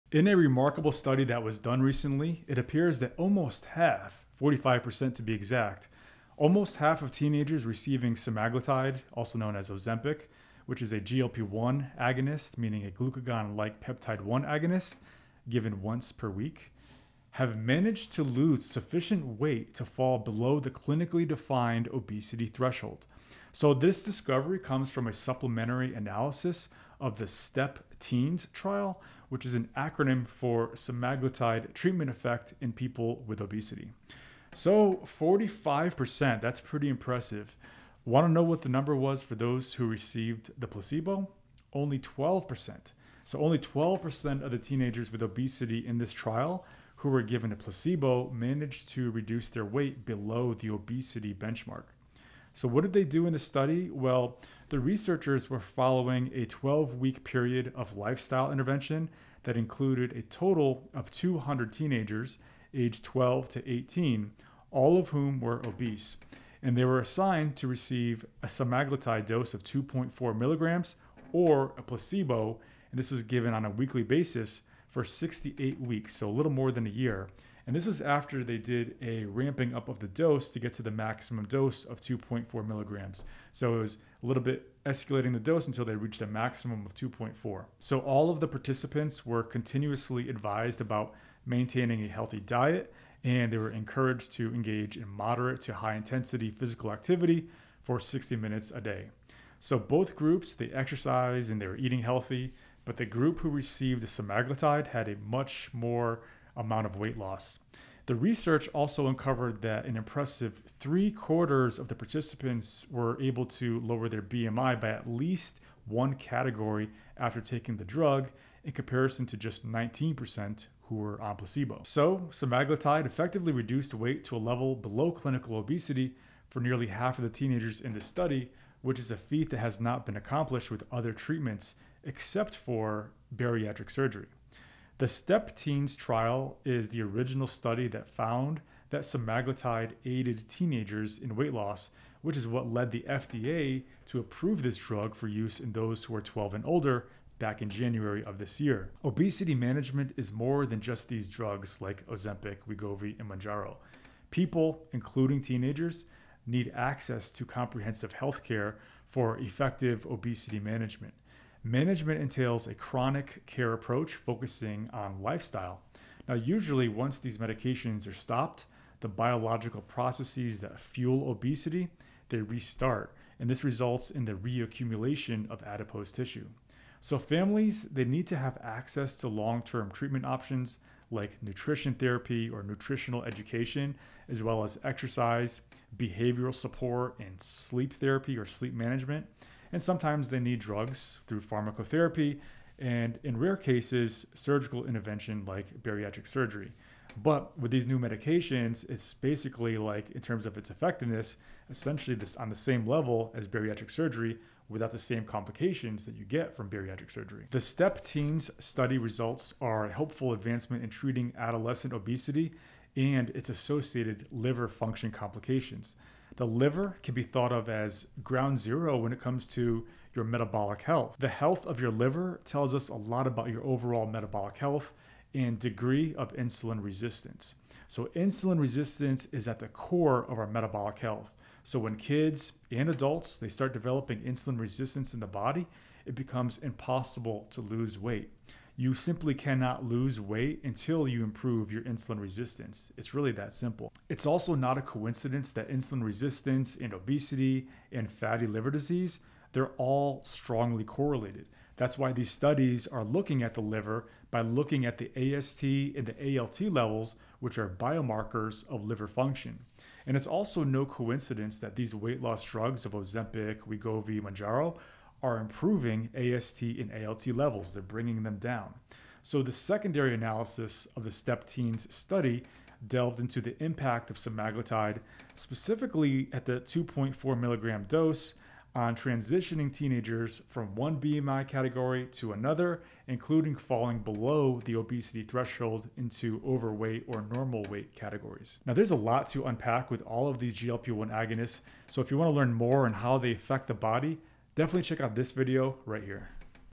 Half of Teens Drop Below Obesity Cutoff With Semaglutide
Semaglutide: A Breakthrough in Teenage Obesity Management - STEP TEENS Study.Discover the groundbreaking findings of the STEP TEENS study, highlighting the remarkable impact of semaglutide in managing teenage obesity. In this video, we delve into the exciting results that showcase how semaglutide, a GLP-1 agonist, helped nearly half of the participating teenagers achieve weight loss below the clinically defined obesity threshold. With a detailed analysis of the trial's methodology and outcomes, we explore how semaglutide outperformed the placebo, with 45% of teens experiencing significant weight reduction compared to only 12% in the control group. Additionally, we uncover the impressive transition of participants from one BMI category to another, with 75% successfully lowering their BMI by at least one category. Join us as we discuss the vital role of semaglutide in tackling teenage obesity and improving metabolic health.